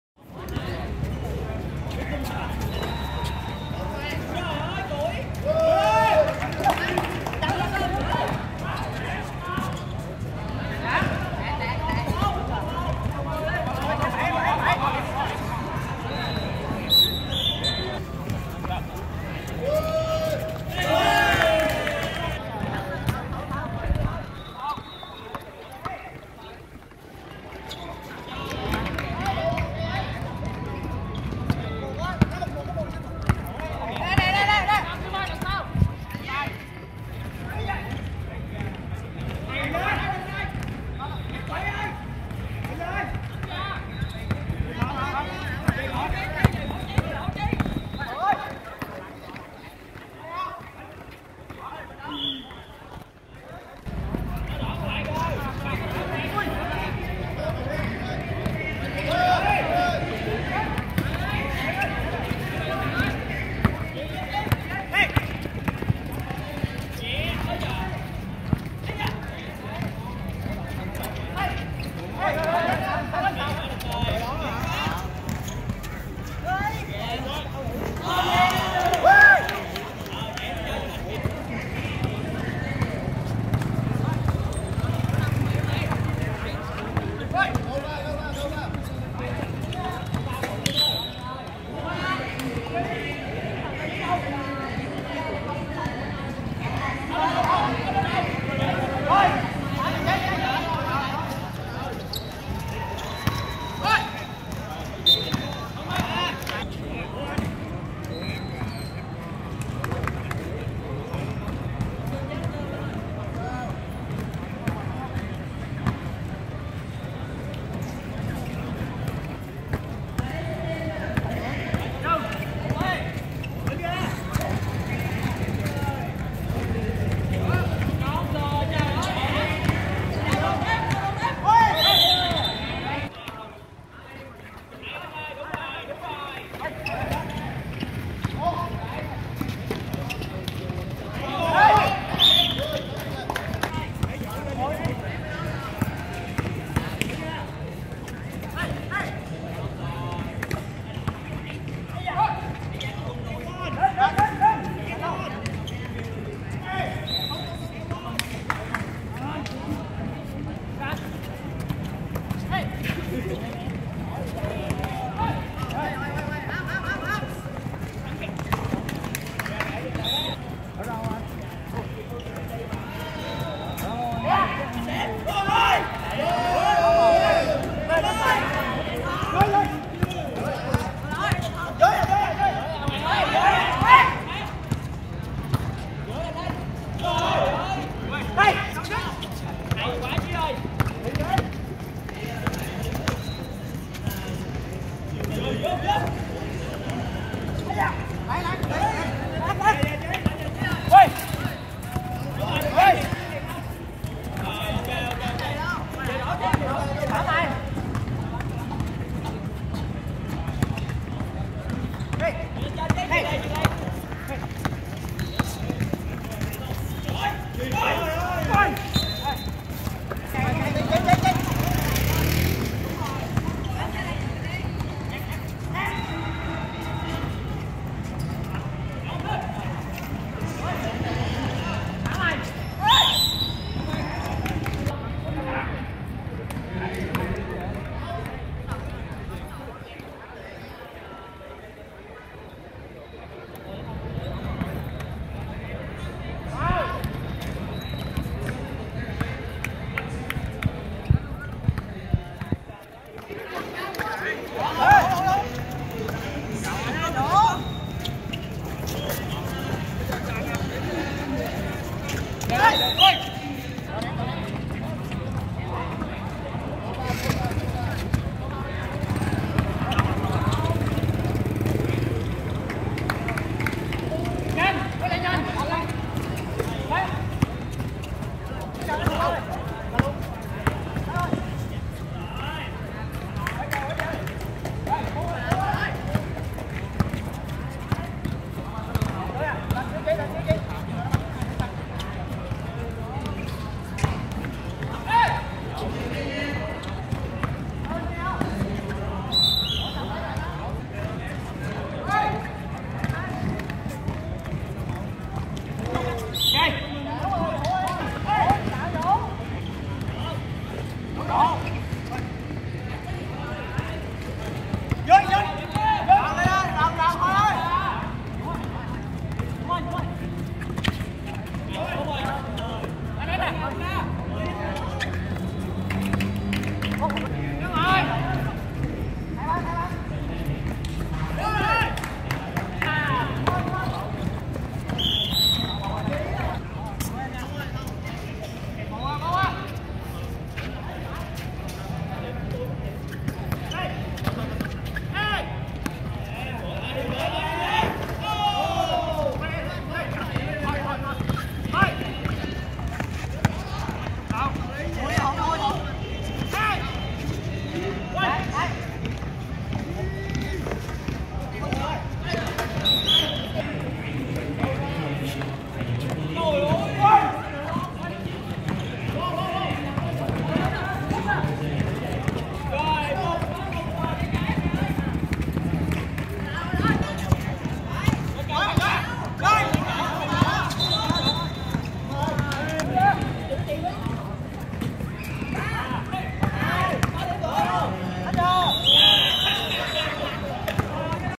Saigon Basketball League ss4 - Quarterfinal 3 - Gò Vấp [Black] 64 W 32 STU [White]
MVP: Gò Vấp - #9 Linh - 18pts + 4rebs + 2asts
Overall Stats:
1. Rebound: Gò Vấp - 13 [#4 và #9 - 4 rebs]; STU - 16 [#5 - 5 rebs]
2. Steal: Gò Vấp - 5 [#4 - 3 stls]; STU - 3 [#9 - 2 stls]
3. Assist: Gò Vấp - 6 [#7 và #9 - 2 asts]; STU - 1 [#23]
4. Block: Gò Vấp - 5 [#4 và #5 - 2 blks]; STU - 1 [#23]